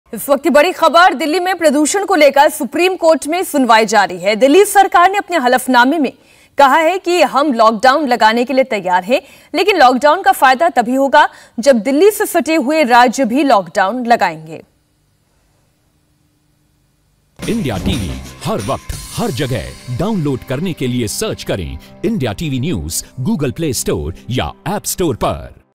दिल्ली में लगेगा लॉकडाउन? Air Pollution को लेकर आज Supreme Court मे चल रही अहम सुनवाई
दिल्ली-एनसीआर में वायु प्रदूषण को लेकर आज सुप्रीम कोर्ट में अहम सुनवाई हो रही है। इस दौरान दिल्ली की केजरीवाल सरकार ने सुप्रीम कोर्ट को बताया कि वह राष्ट्रीय राजधानी में वायु प्रदूषण से लड़ने के लिए लॉकडाउन जैसे कदम उठाने के लिए तैयार है। हालांकि सरकार ने कहा कि इसका सीमित प्रभाव ही होगा। उन्होंने कहा कि ऐसा कदम तब सार्थक होगा जब इसे एनसीआर क्षेत्र के अलावा पड़ोसी राज्यों में भी लागू किया जाए।